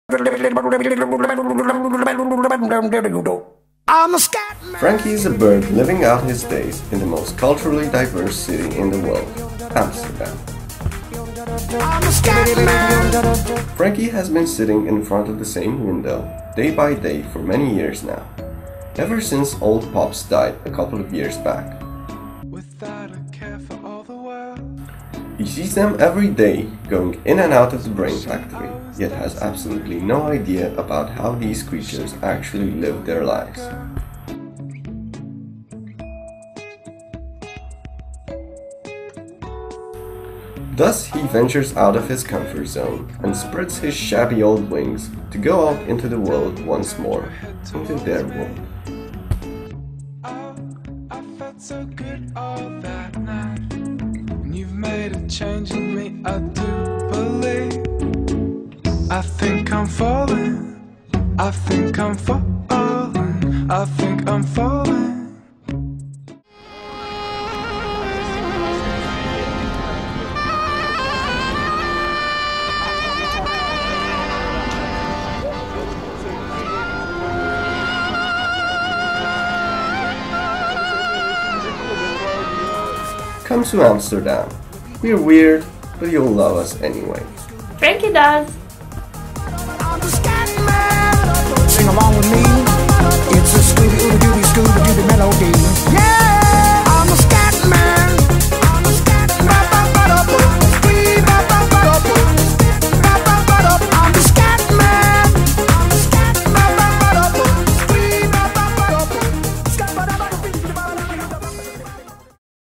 Frankie - A bird's eye view to Amsterdam (short version)
A different perspective on Amsterdam by Asandei Alexandru and Cristea Tatiana